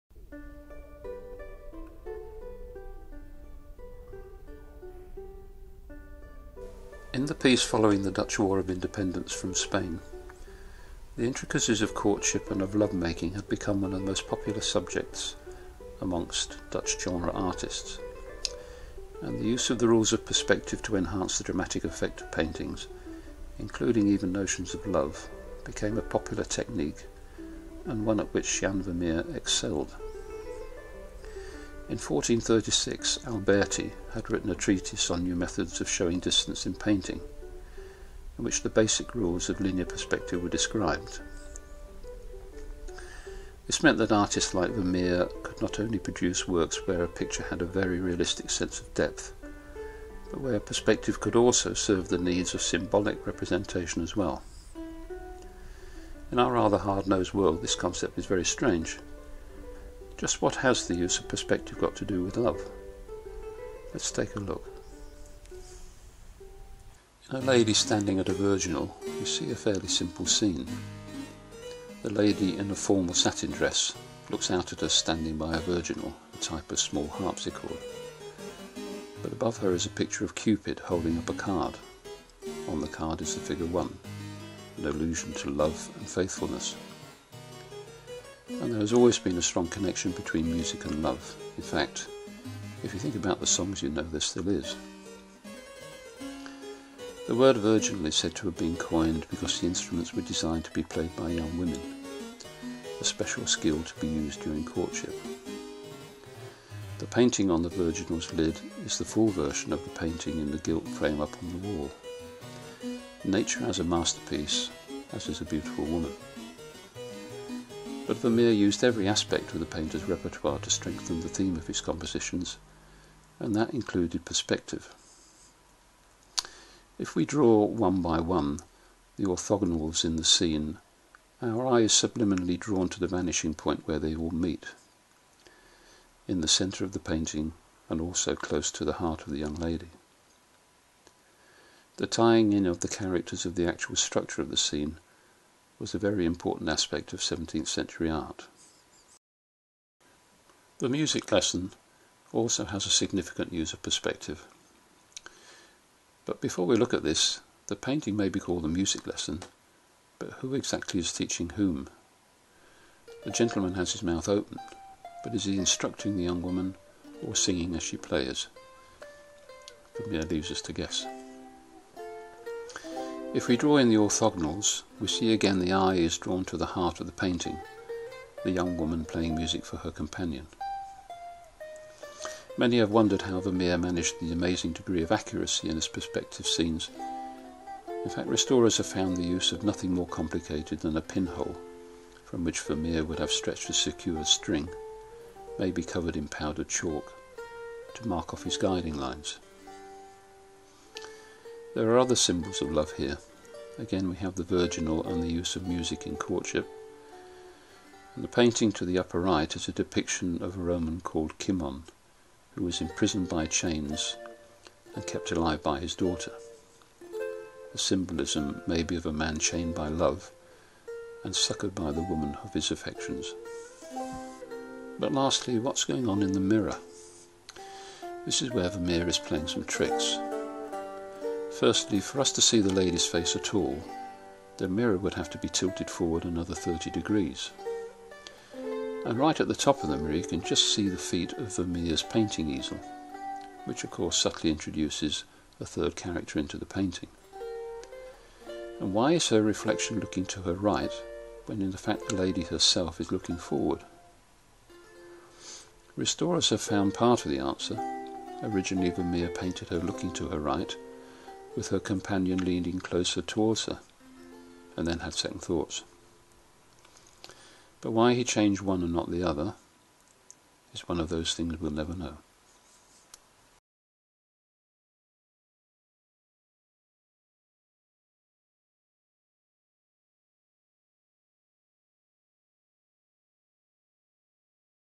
Vermeer and Love in Perspective
Jan Vermeer's use of perspective in works of love and courtship.including:  A lady standing at a Virginal, The Music Lesson, The Love Letter and The Glass of Wine.